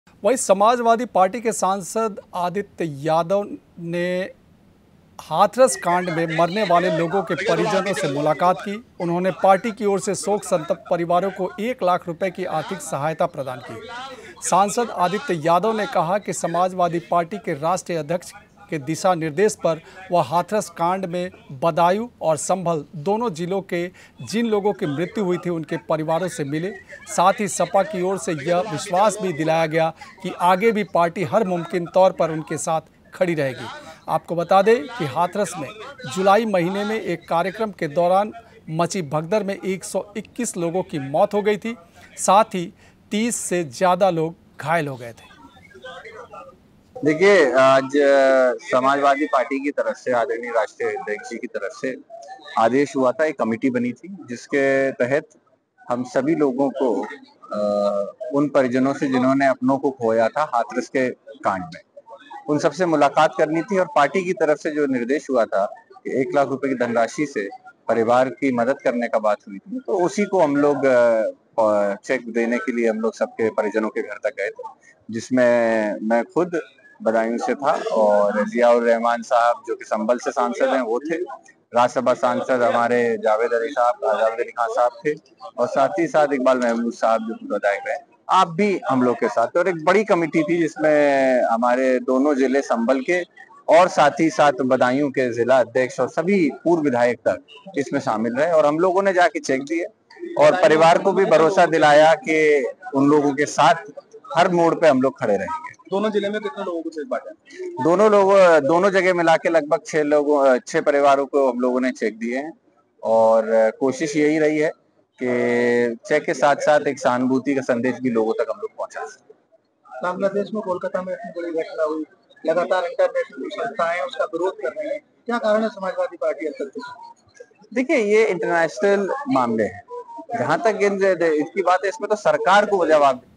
सपा सांसद ने दी एक-एक लाख रुपये की मदद
हाथरस हादसे के मृतकों के परिजनों से मिले सांसद
बदायूं और संभल के लोगों की हुई थी मौत
हाथरस में जुलाई में हुई थी भगदड़
भगदड़ में 121 लोगों की गई थी जान